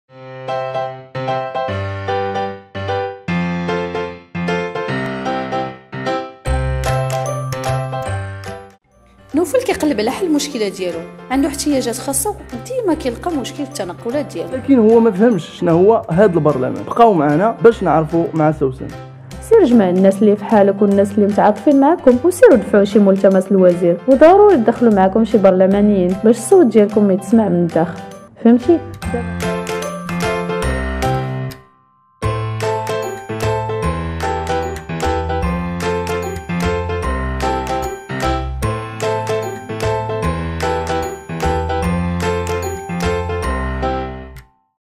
Li Tgal Yddar : Vidéo promotionnelle
« Li Tgal Yaddar » est un projet mené par l'association Racines en partenariat avec la Fondation Heinrich Böll Afrique du Nord Rabat et financé par le ministère des affaires étrangères de la république allemande. ). Il fait suite au projet «Accompagnement des préparatifs des élections 2015 en vue d’une démocratie participative et responsable » mené entre septembre 2014 et février 2015 et dont la mission principale est d’accompagner les élections locales, régionales et parlementaires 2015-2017.

"لي تڭال يدار"، هو مشروع تقوم به جمعية جذور بشراكة مع مؤسسة هينريش بول شمال إفريقيا الرباط ويستفيد من دعم وزارة الشؤون الخارجية لجمهورية ألمانيا الفدرالية. هذا المشروع هو تتمة لعملية مواكبة تحضيرات انتخابات 2015 من أجل ديموقراطية تشاركية ومسؤولة، والذي أجري بين شتنبر 2014 وفبراير 2015 والهدف منه هو دعم الانتخابات المحلية، الجهوية والبرلمانية 2015ـ2017.